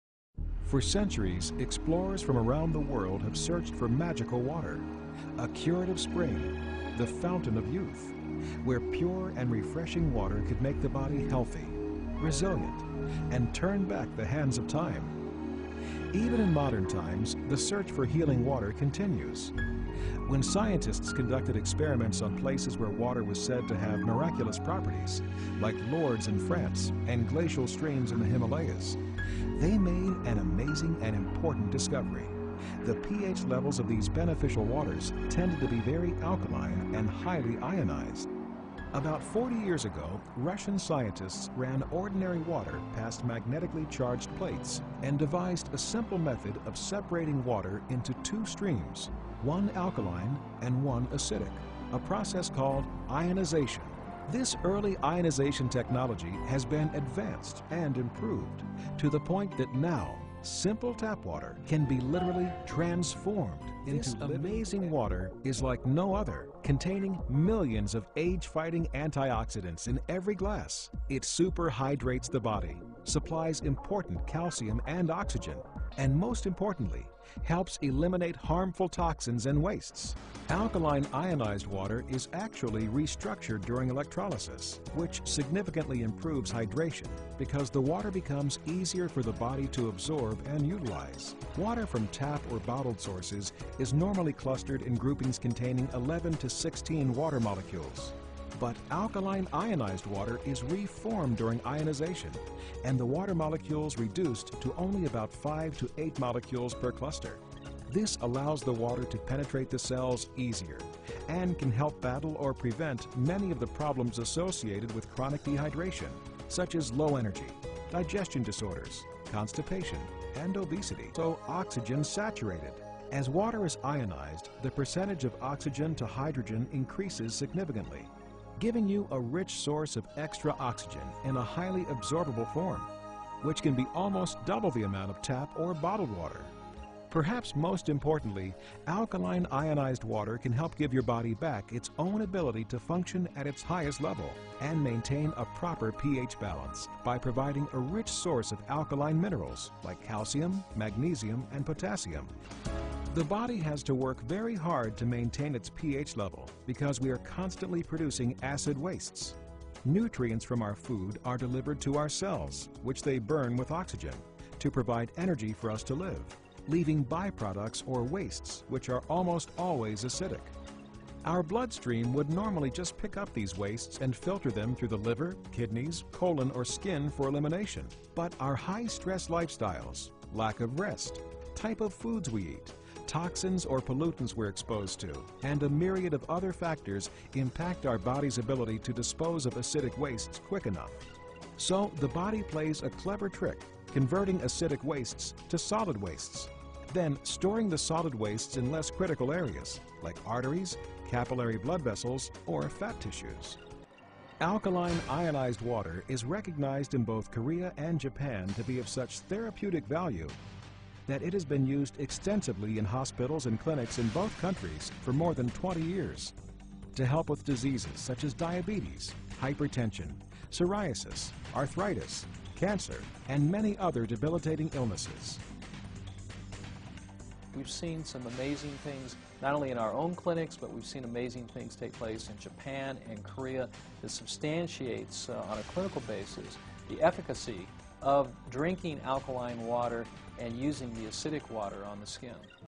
Alkanlysed Anti Oxidant & Kangen Water Diabetics Treatments
This video shoes the properties of Alkanlysed, Anti-Oxidant and Kangen Water used for diabetic patient treatment in Japan.